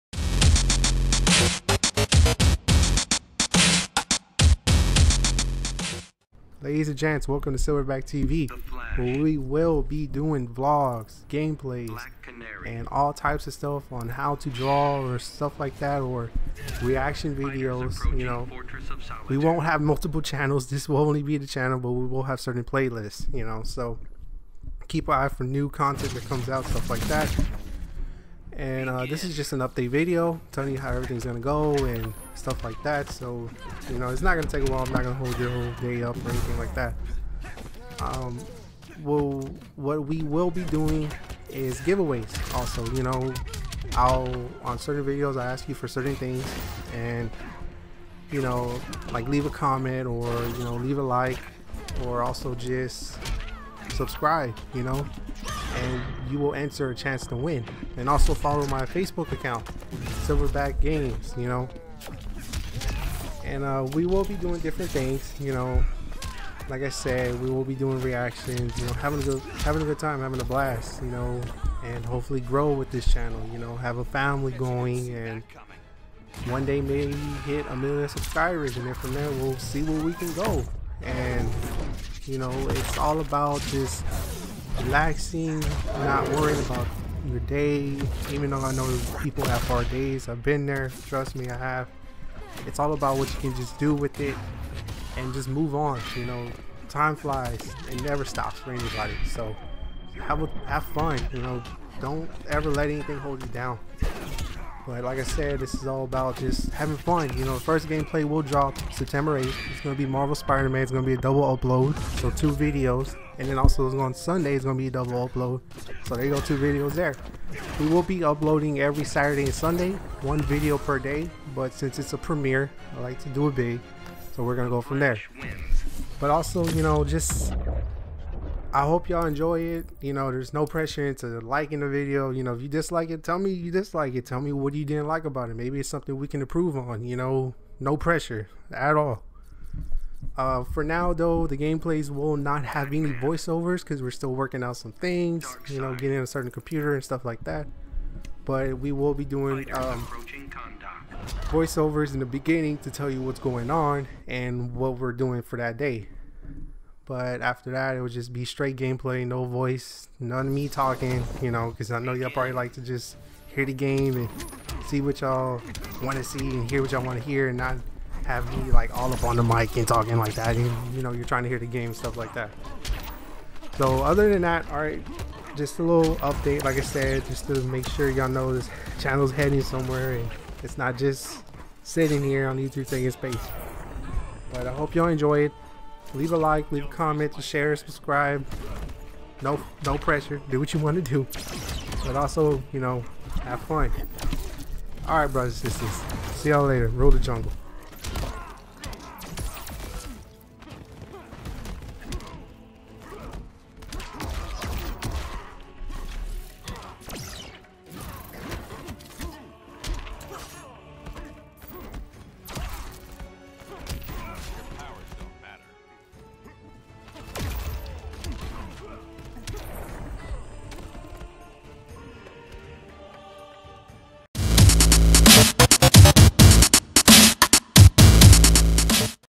introducing Silverback TV!!!!
just an update on the channel guys on how everything is going and what this channel will be about.